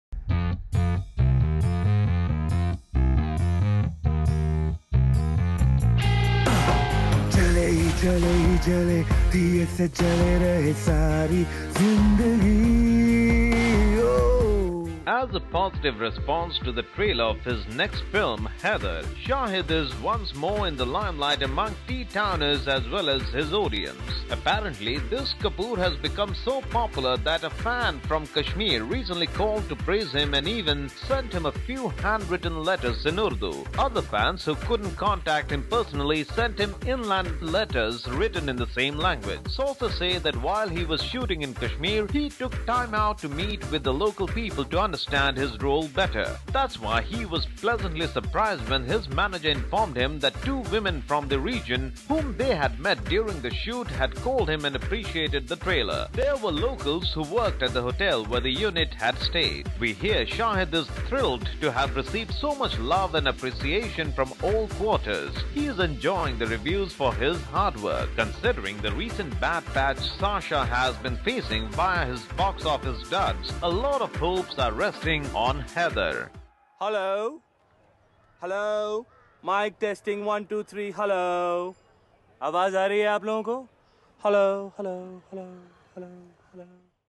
Not so secret admirers | Bollywood Masala | Latest Bollywood News
Watch Latest Bollywood Masala News As a positive response to the trailer of his next film, Haider, Shahid is once more in the limelight among T'Towners as well as his audience. Apparently, this Kapoor has become so popular that a fan from Kashmir recently called to praise him and even sent him a few handwritten letters in Urdu. Other fans, who couldn’t contact him personally, sent him inland letters written in the same language.
Sources say, “While he was shooting in Kashmir, he took time out to meet with the local people to understand his role better. That’s why he was pleasantly surprised when his manager informed him that two women from the region, whom they had met during the shoot, had called him and appreciated the trailer. These were the locals who worked at the hotel where the unit had stayed.”
We hear Shahid is thrilled to have received so much love and appreciation from all quarters. He is enjoying the reviews for his hard work. Considering the recent bad patch Sasha has been facing via his box-office duds, a lot of hopes are resting on Haider.